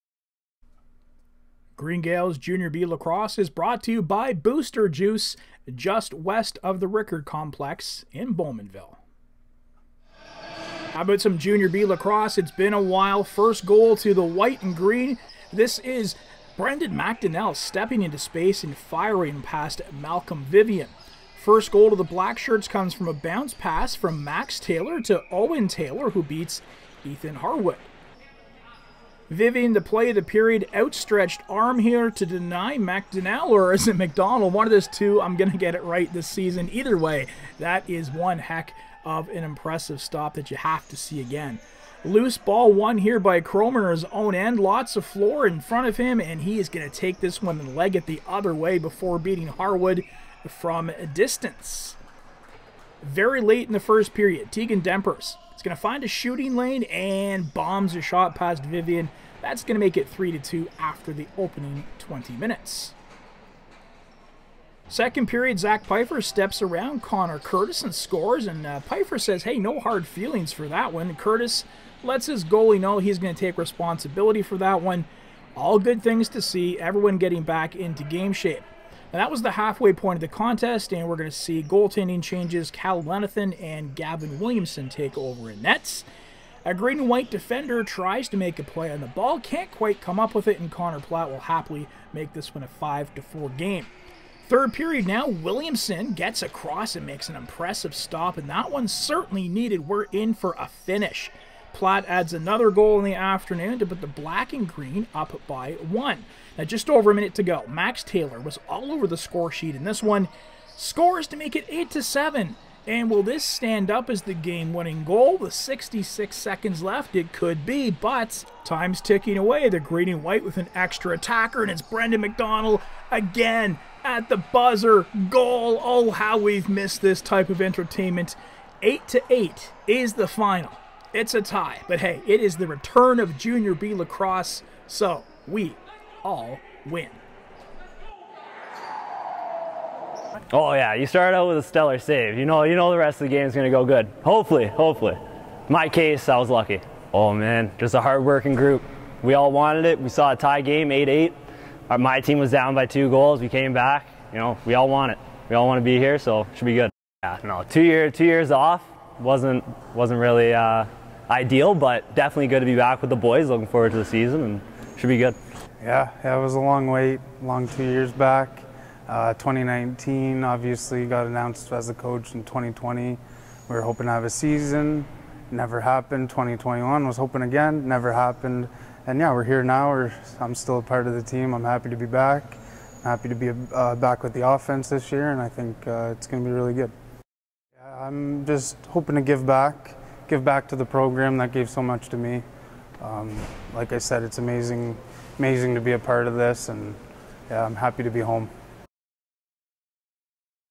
Green Gaels Jr B Lacrosse Green and White vs Green and Black
The Green Gaels return to the floor with an exhbition game
It's the Green and Black vs The Green and White